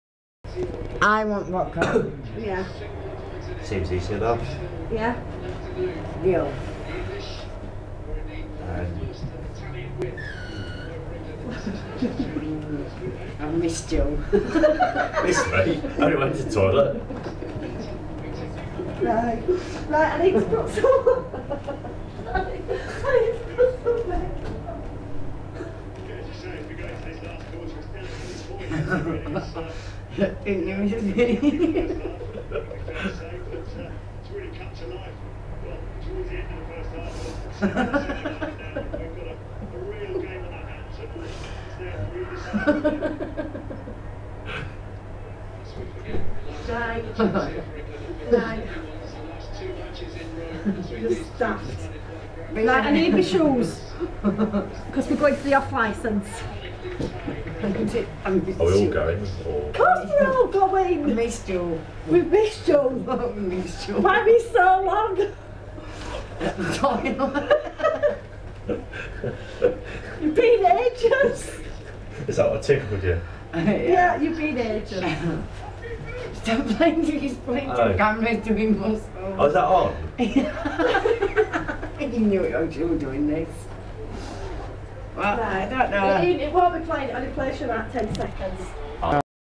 comedy clip... how to get a boyfriend
must watch. i missed you. poor lad we only just met him, he must have thought we were crazy. met him in manchester, we were there for big big brother auditions, ended up going roung manchester, had a right laugh